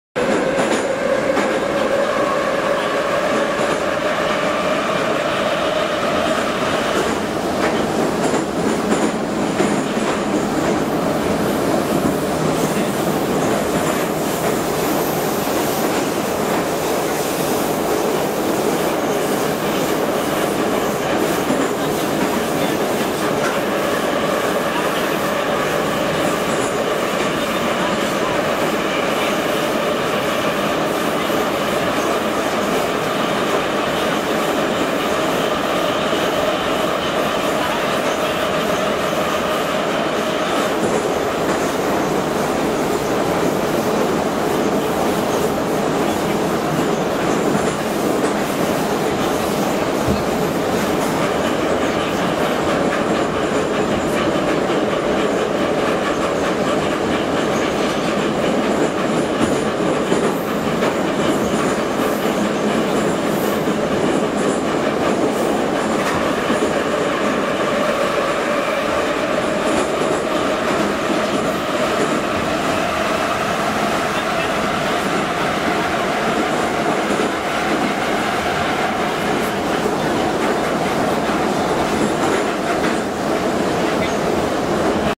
Замер радиации в метро
Замер мощности дозы гамма-излучения в тоннеле Московского метрополитена между станциями Третьяковская и Китай-Город.
Хотя малое время замера не даёт получить точные данные, однозначно можно сказать, что уровень радиации в этом тоннеле в разы ниже, чем на поверхности.